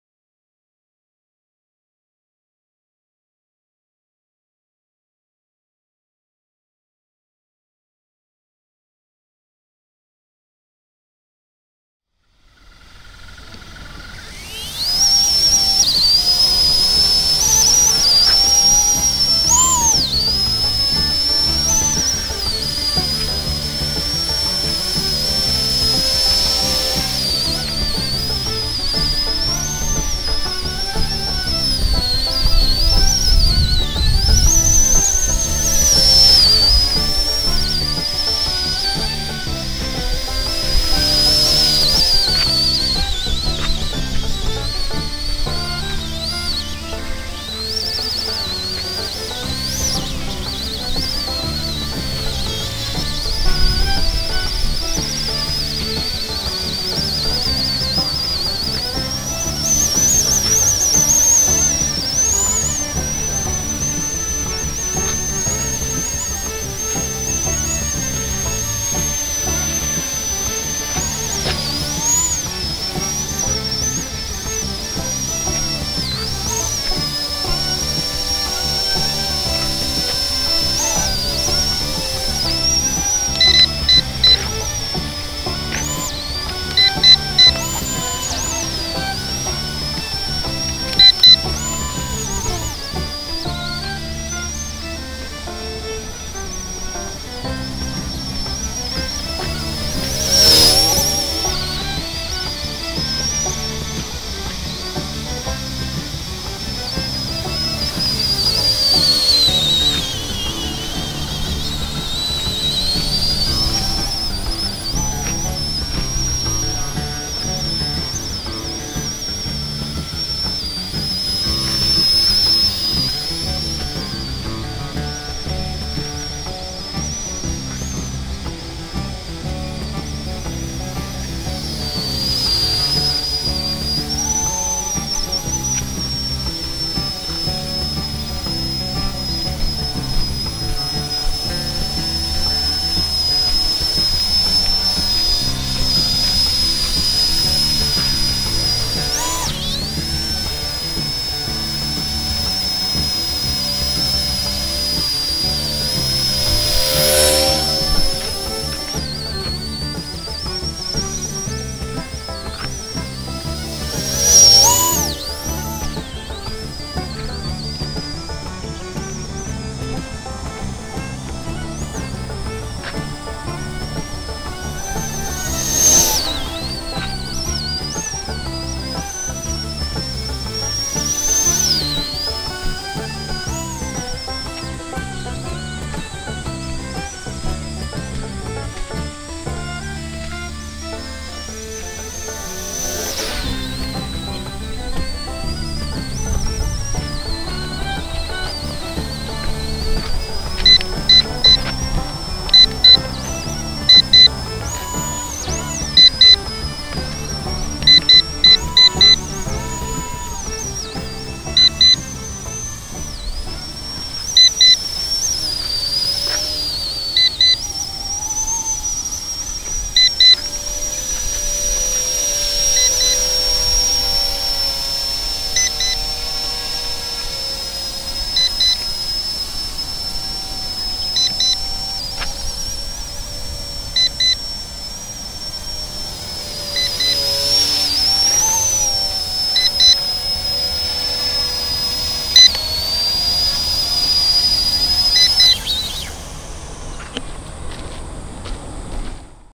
20240705 c123
C123 EC135　3D飛行はできませんが近年小型スケールヘリの中で最高の出来だと思いました。でもちょっと値段が…　いい機体なんですがRC ERAの中では間違いない機体だと思います。